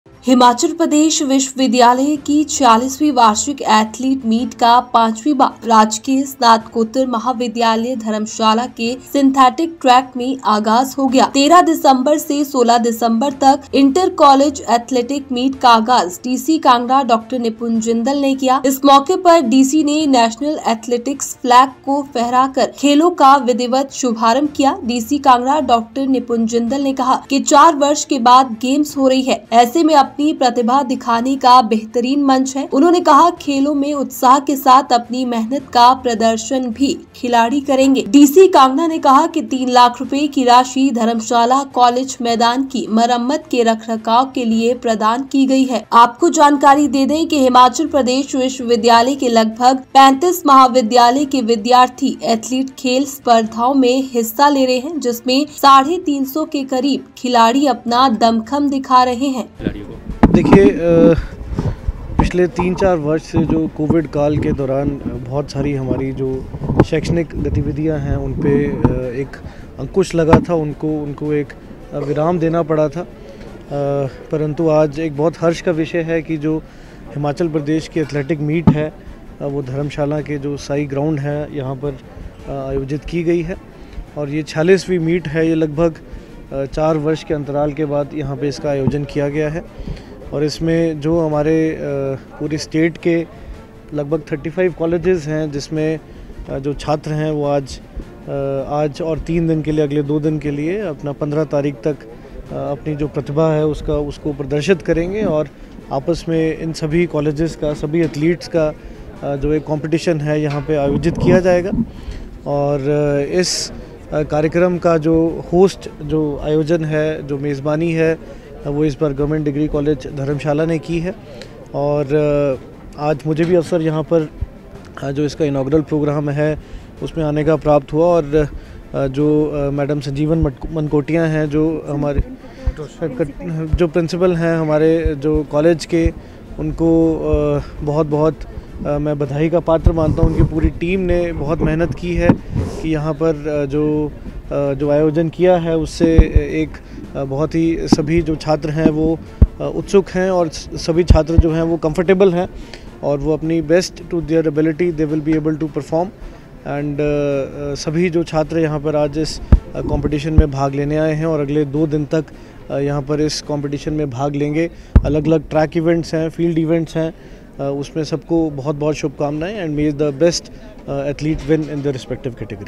हिमाचल प्रदेश विश्वविद्यालय शिमला की 46वीं वार्षिक एथलीट मीट का आगाज... | Himachal | Latest Updates
हिमाचल प्रदेश विश्वविद्यालय शिमला की 46वीं वार्षिक एथलीट मीट का आगाज.

46th annual athlete meet of Himachal Pradesh University, Shimla begins.